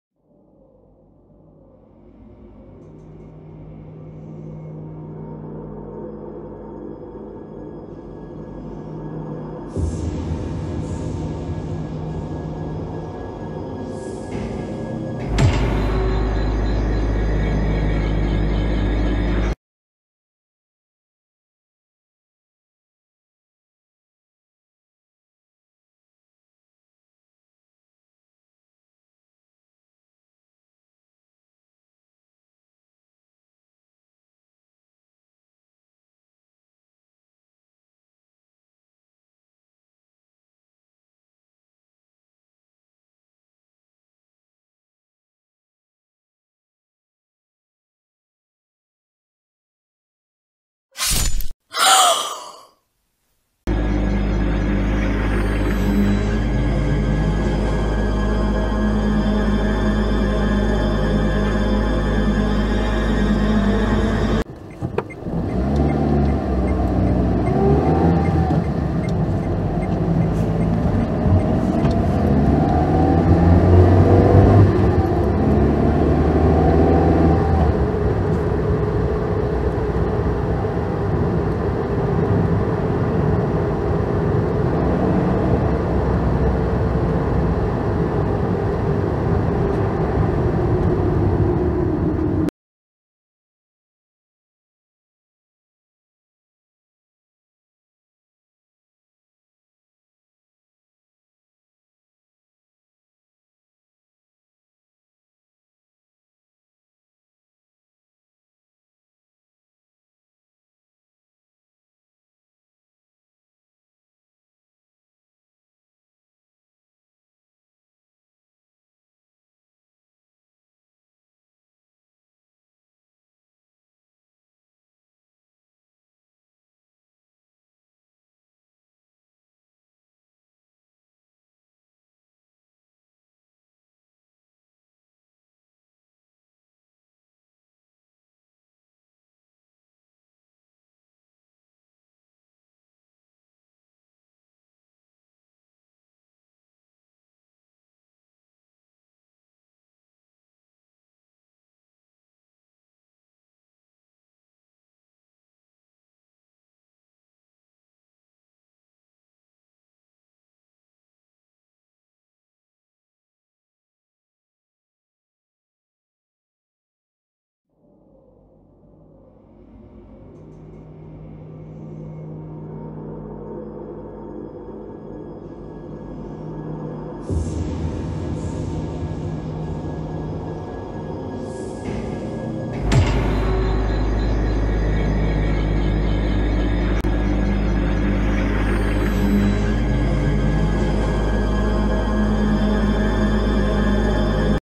The Reunion Full Comic Part 1
part 1 of an original horror comic straight from the scary side of the mind of Ryan Melrose.. enjoy